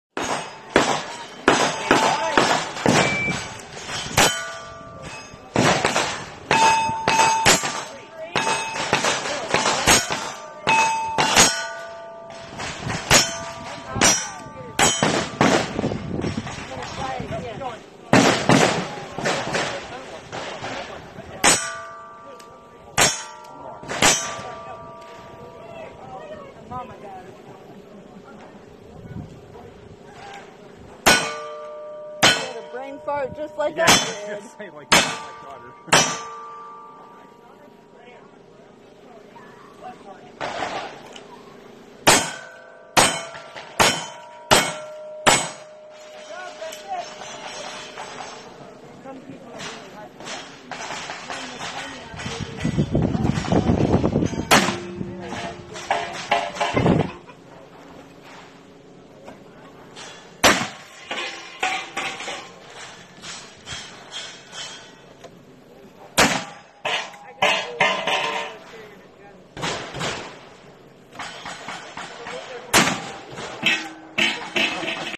evils little angel gunsmoke 2022 stage 7
9 year old girl competing in sass